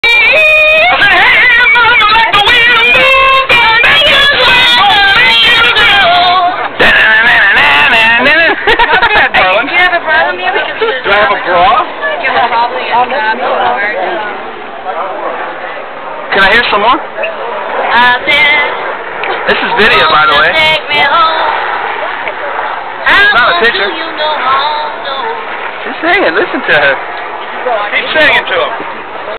Meatloaf Sings Led Zeppelin
At the Dan Marino CGA Weston Hills CC Meatloaf has some fun with us while on the back 9.